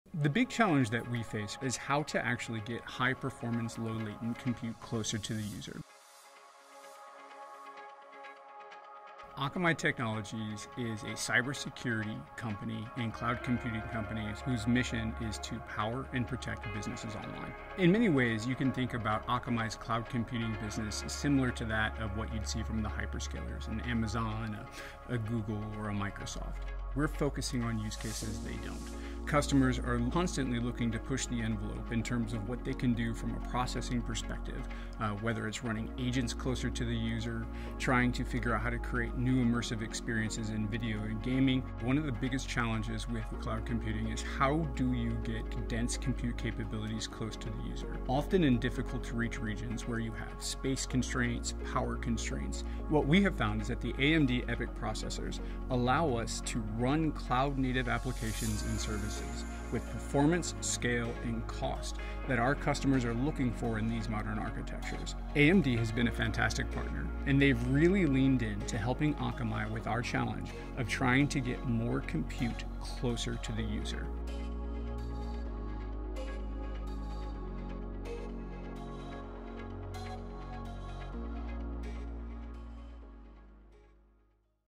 Akamai brings compute closer to users with AMD EPYC™ CPUs
What the video to learn how Akamai brings high-performance cloud compute closer to users by deploying AMD EPYC™ CPUs for scalable, cost-efficient edge infrastructure.